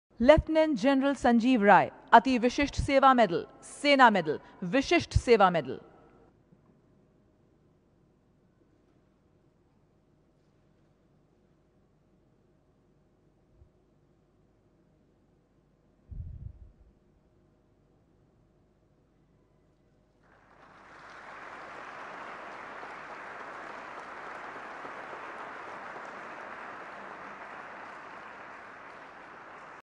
President Kovind presents Param Vishisht Seva Medal to Lieutenant General Sanjiv Rai
President Kovind presents Param Vishisht Seva Medal to Lieutenant General Sanjiv Rai, AVSM, SM, VSM, Infantry.